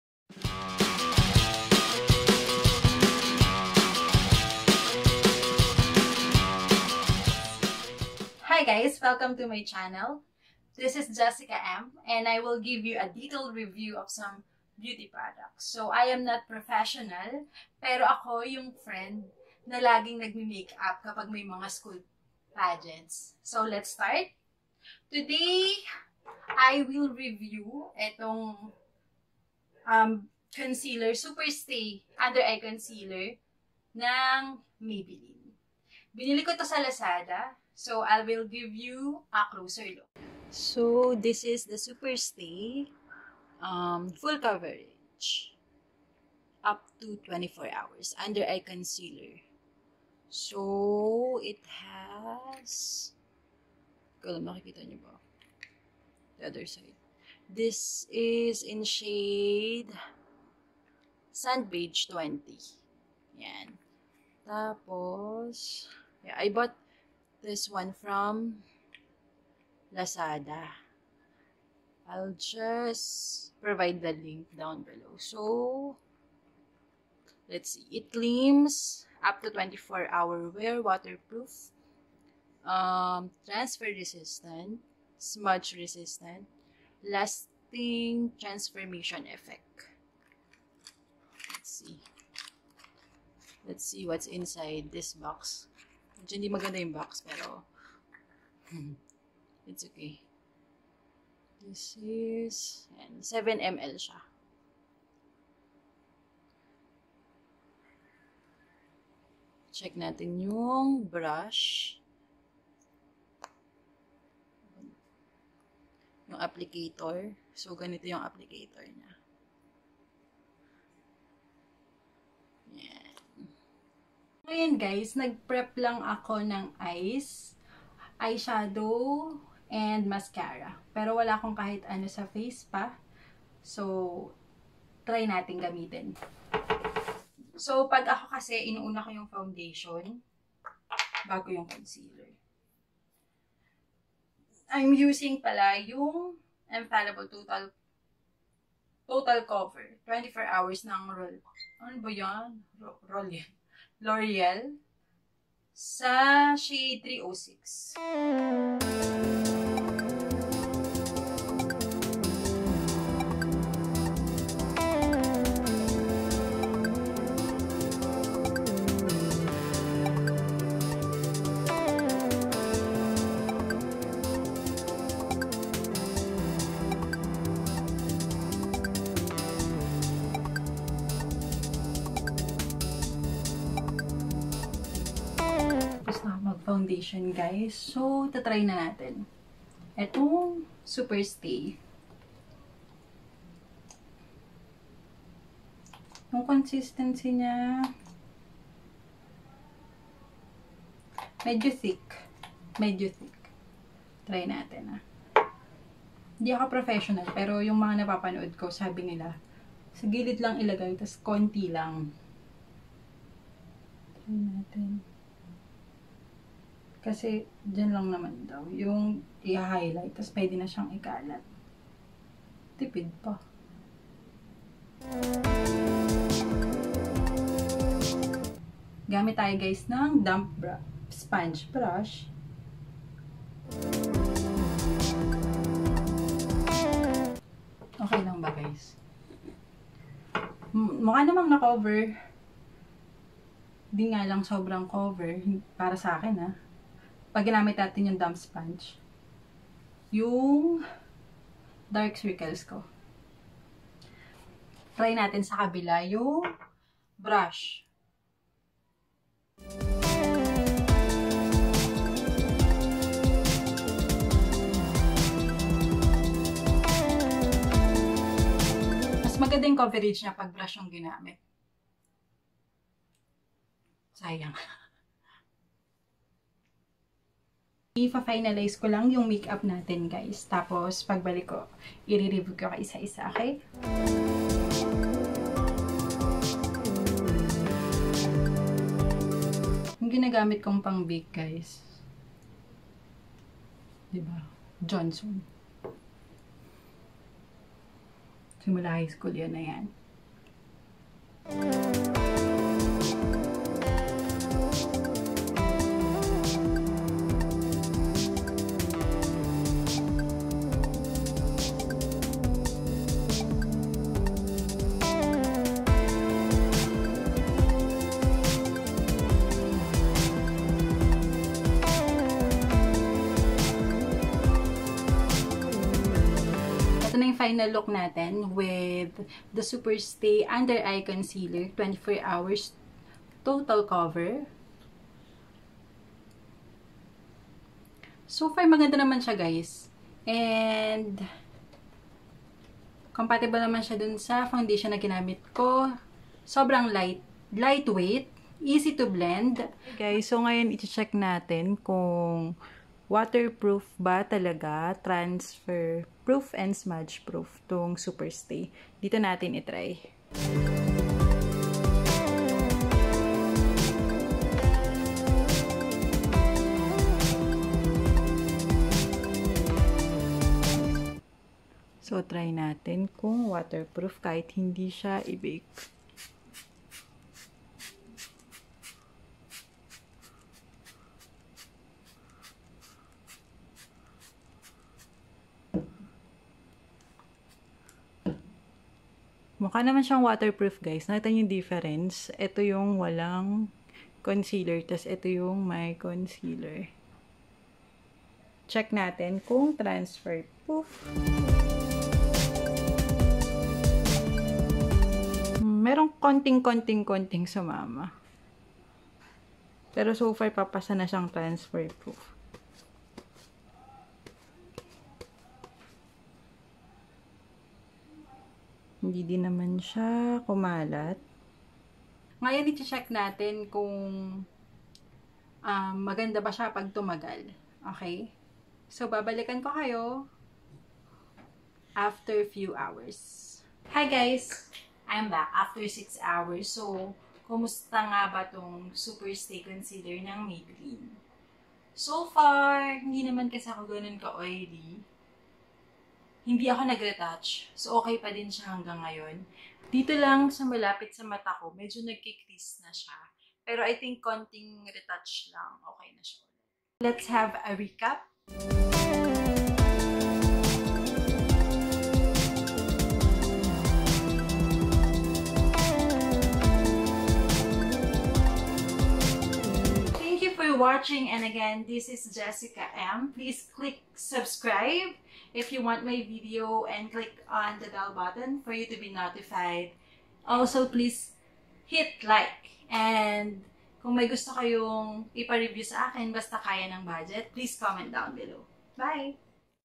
Superstay 24H Full Coverage Concealer Review
Hi mga bebelabs, here's a detailed and honest review of this Concealer from Maybelline

Product used:
Superstay 24H Full Coverage Concealer Makeup by Maybelline